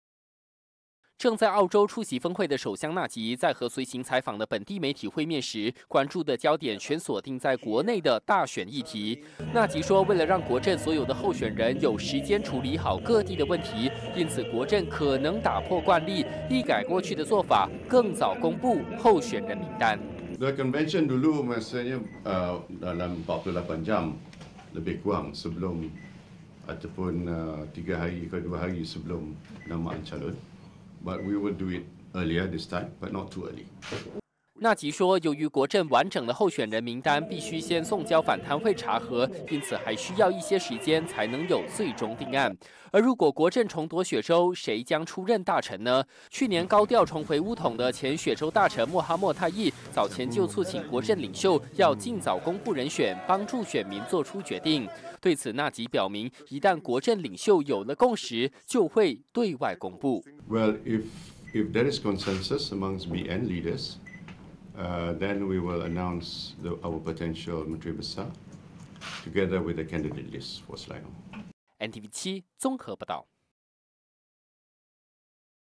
破例迎大选 国阵或提早公布候选人名单
大选气氛越来越浓，各政党的候选人陆续揭晓。首相拿督斯里纳吉透露，国阵这次或许会改变做法，更早公布候选人名单，好让候选人能有充分时间做好迎战的准备。如果真的提早公布，意味着国阵将打破以往惯例，来迎战本届大选。

首相兼国阵主席拿督斯里纳吉：”之前的惯例是在大约48小时前，或是提名日前两三天，但我们这次会提早，但也不会太早。如果国阵领袖间能达成共识，我们就会在宣布雪州候选人时，一并宣布潜在的大臣人选。“